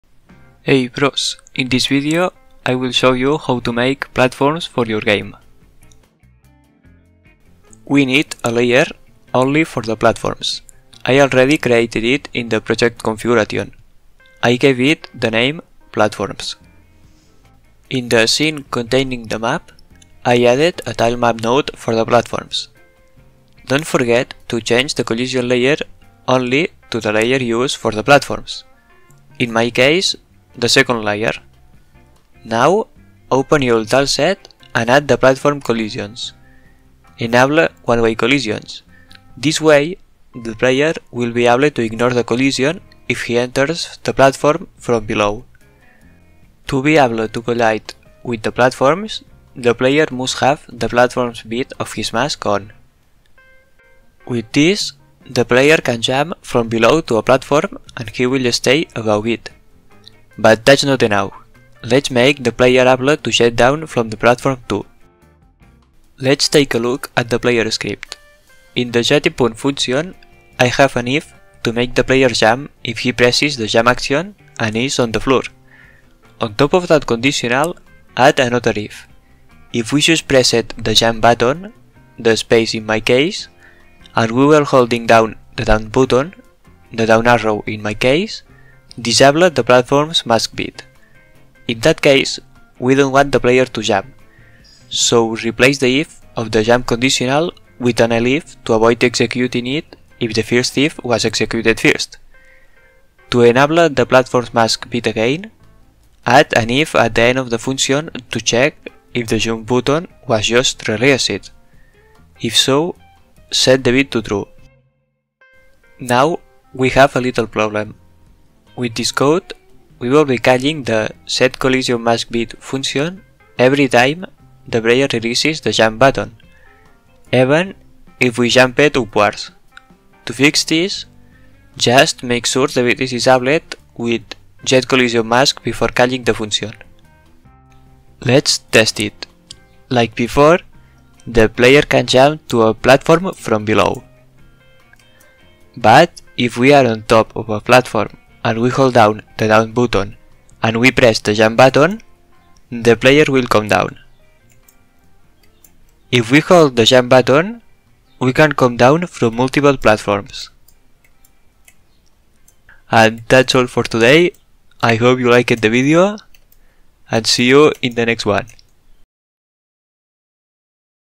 How to make platforms in Godot
In this video we will see how to make platforms for our character. After the video, the player will be able to get up to a platform jumping from below, and get down pressing the jump button when holding the down button.

Tip: on the tilemap editor, you can rotate the tiles with 'a', 's', or the icons at the top of the tile list.

0:00 Intro
0:07 Platforms layer
0:17 TileMap and one way collision tiles
0:53 Test one way collisions
1:05 Jump down
2:18 Final result and end